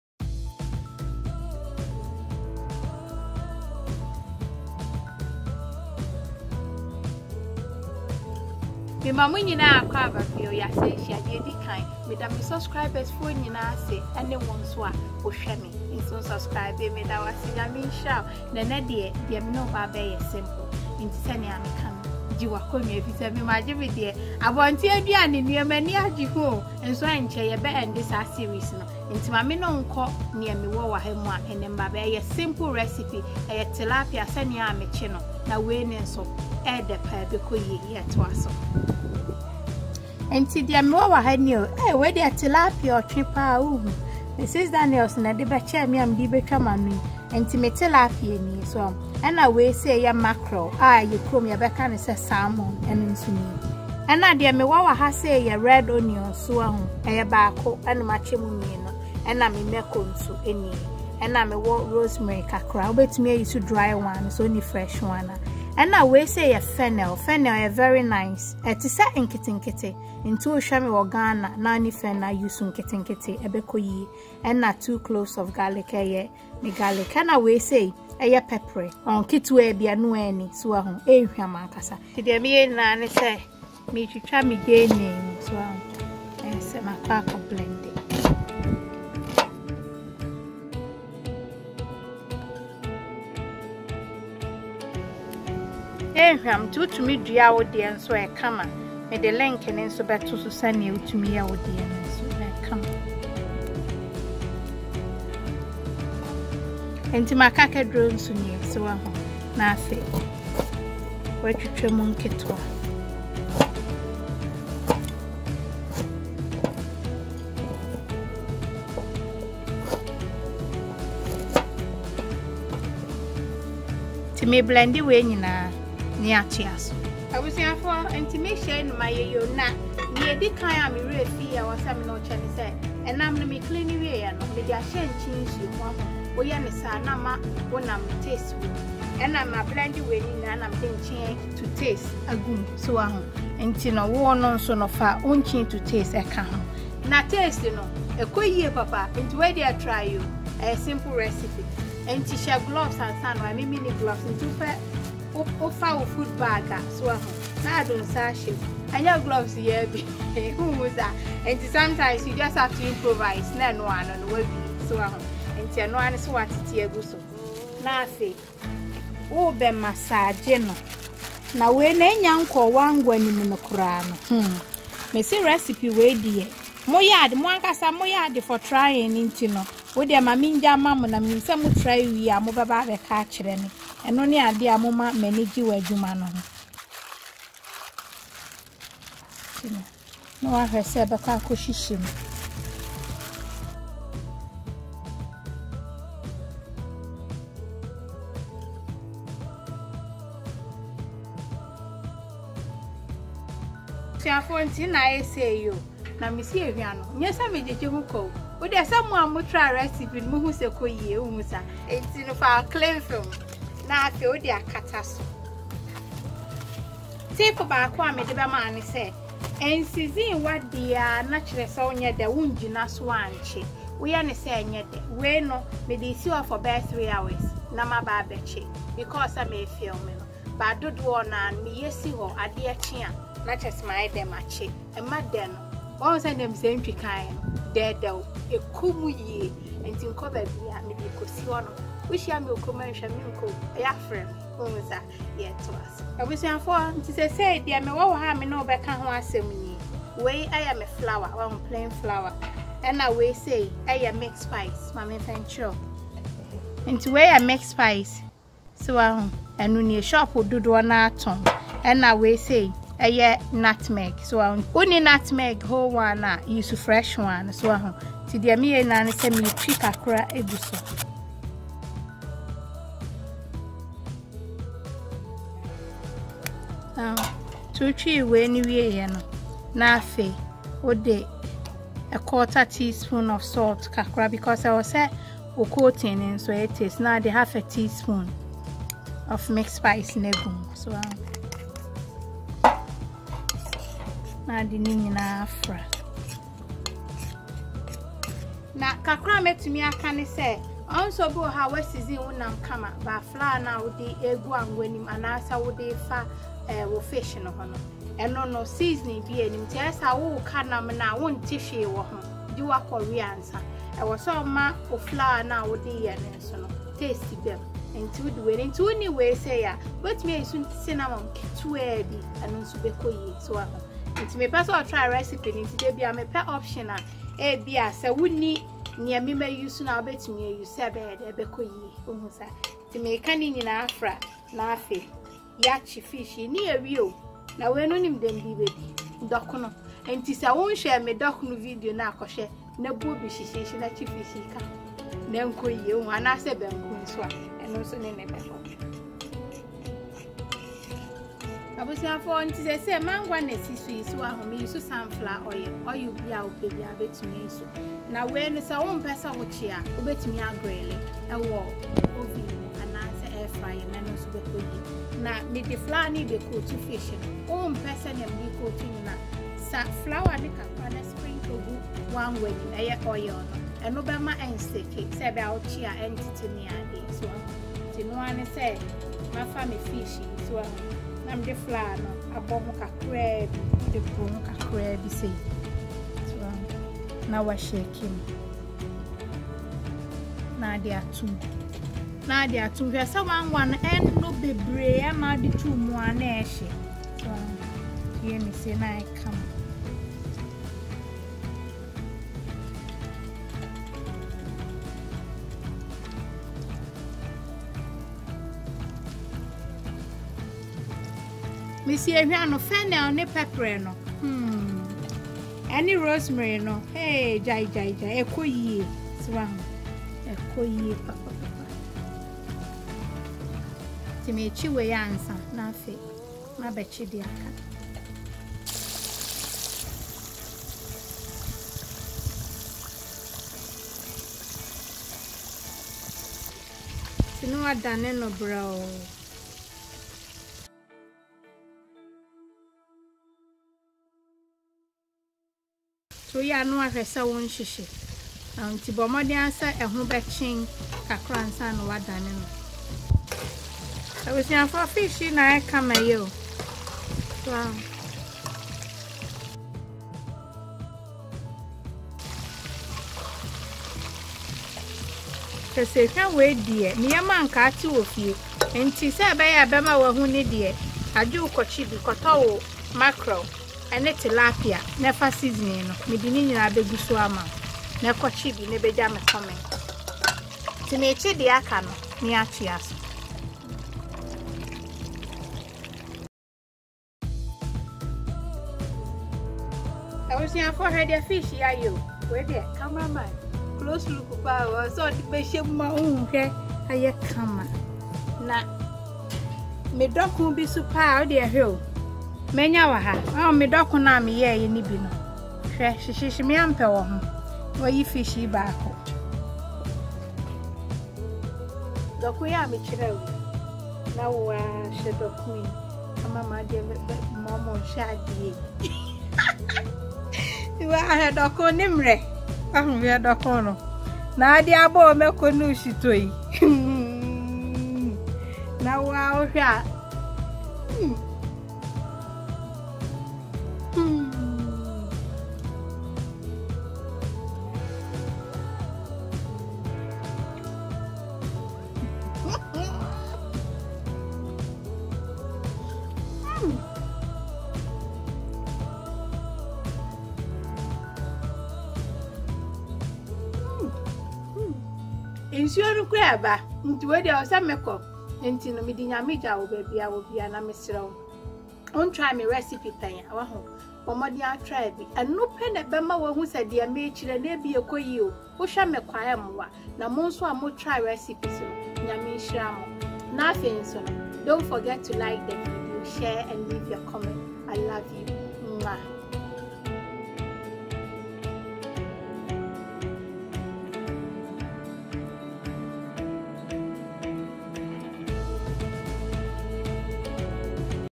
HOW TO FRY A VERY TASTY MACKEREL AND TILAPIA FISH: COOKING IN THE GARDEN WITH MAMA BETTY- EP 11
Ingredient
Tilapia and Mackerel fish, 1 red onion, 2 habanero
pepper, fresh rosemary, fennel, 2 cloves of garlic,
and cloves